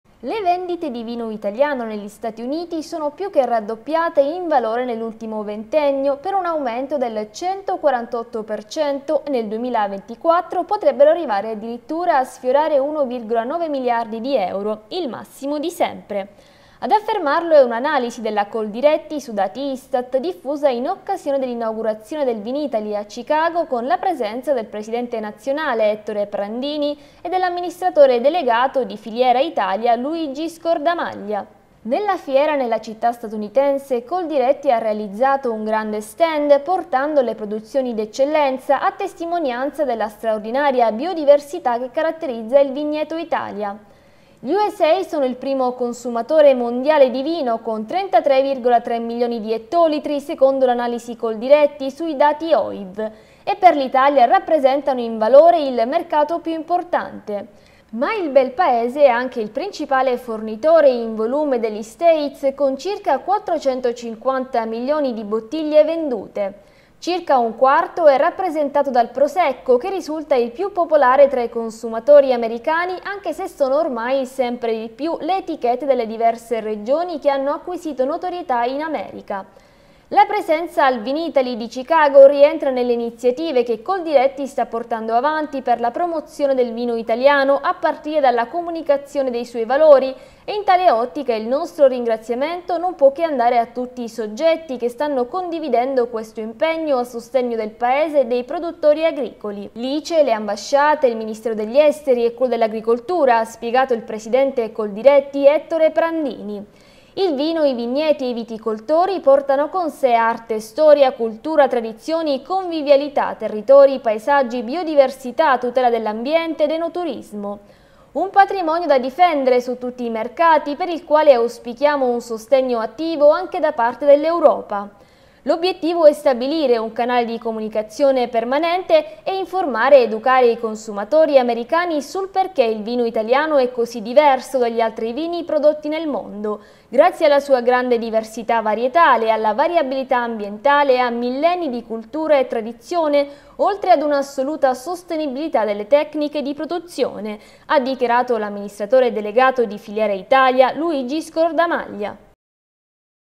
Analisi di Coldiretti: più che raddoppiate le vendite di vino italiano negli Stati Uniti
Analisi di Coldiretti: più che raddoppiate le vendite di vino italiano negli Stati Uniti, +148% in 20 anni